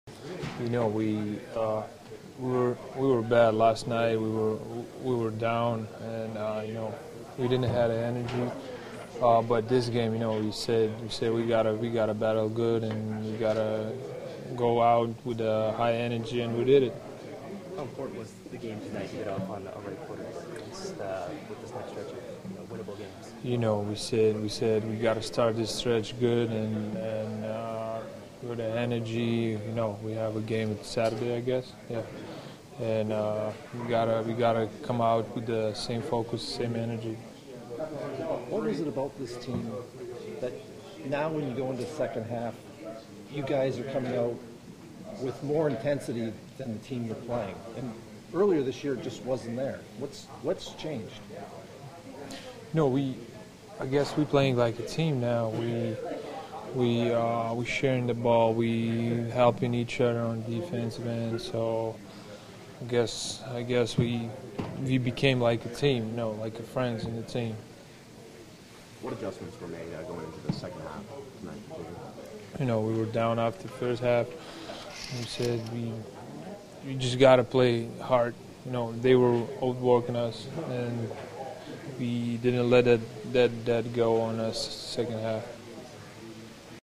Raptors Post-Game: Jonas Valanciuans
Jonas Valanciunas speaks to the media after scoring 16 points and grabbing 11 boards in a win over Detroit. For more post game reaction, go to raptors.com.